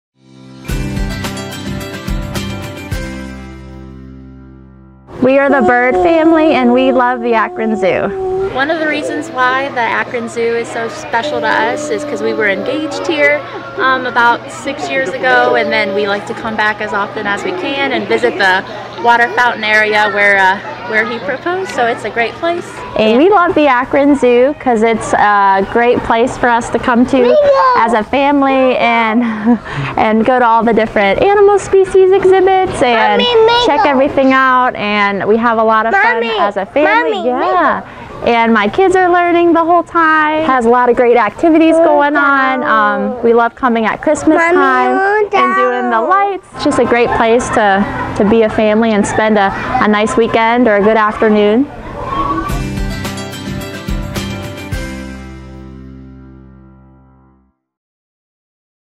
Why I Love the Akron Zoo Contest
We're looking for the next star of our TV commercial. Submit a video telling us why you love the Akron Zoo in 65 seconds or less.